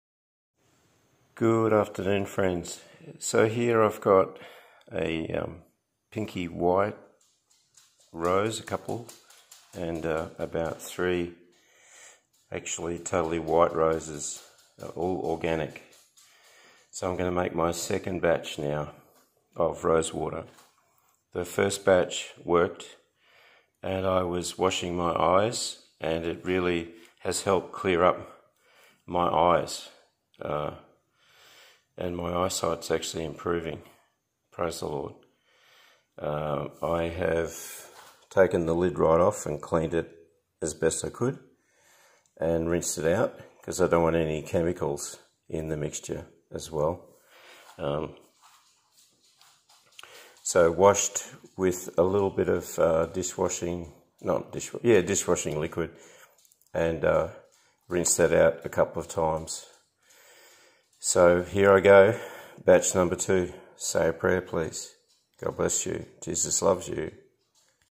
Rose Petal Water Organic Eye Cleanse Batch 2 Part1#flowers eyecleanse organiconly December 6, 2023
2nd batch eye cleanser organic Rose Petal water.